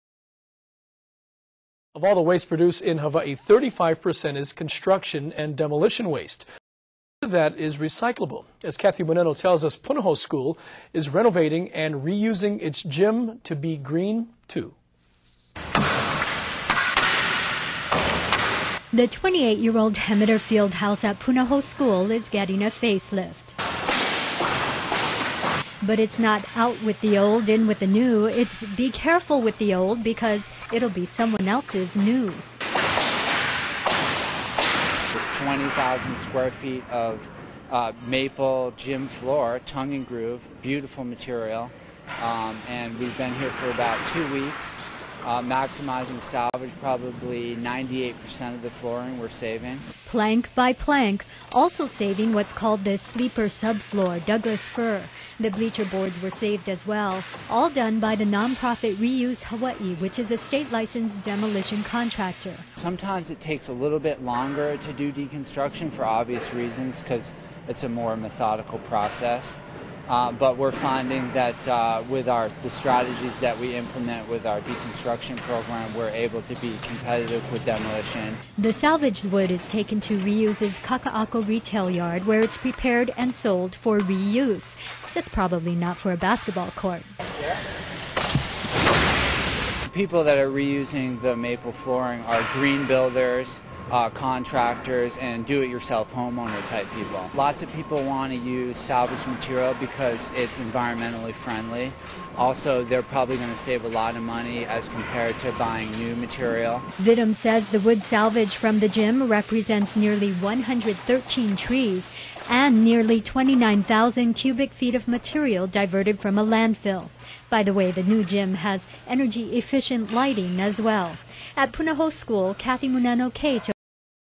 Re-use KHON Coverage
News coverage of Re-use Hawaii deconstructing maple gym floor at Punahou School on Oahu.